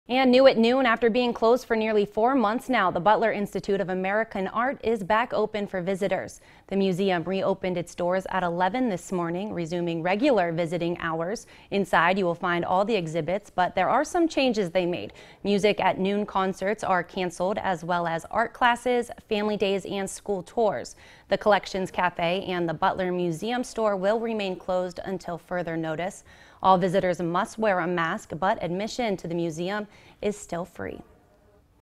The Butler set to reopen Tuesday
The museum has been closed since the pandemic hit in March